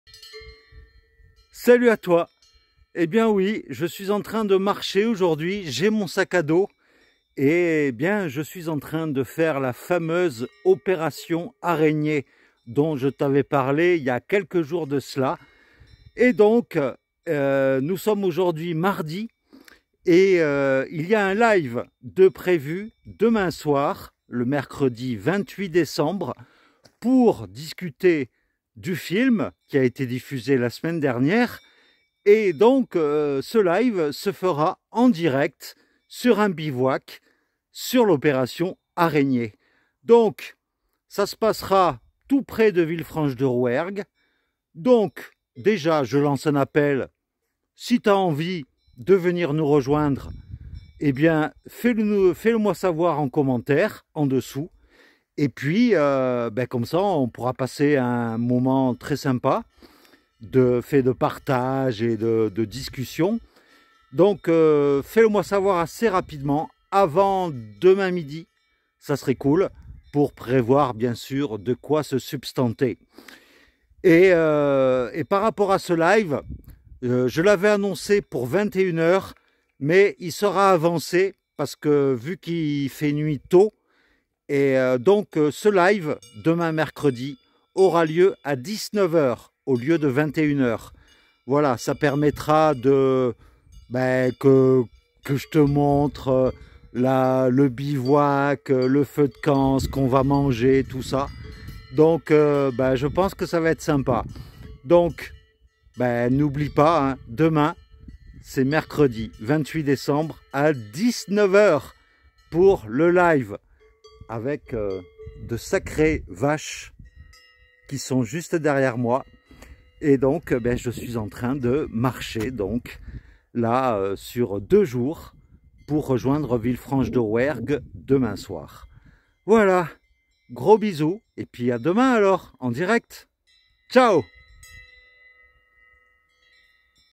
Infos sur le live de demain mercredi 28 décembre !
Rendez-vous demain mercredi 28 décembre à partir de 19h00 pour un live spécial sur le film "VERS COMPOSTELLE ET AU-DELÀ" ainsi qu'en direct de l'opération Araignée 😉